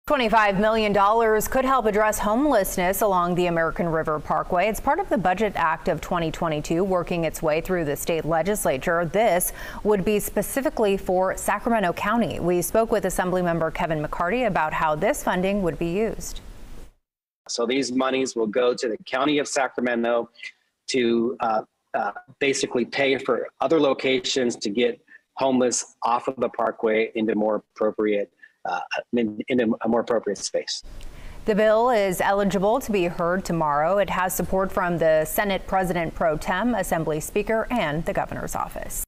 $25M as part of CA Budget Act could go to addressing homelessness
The Budget Act of 2022 is working its way through the State Legislature and $25 million could be used to help address homelessness along the American River Parkway.  The money would be specifically for Sacramento County.  Assemblymember Kevin McCarty spoke with KCRA 3 on how the funding would be used.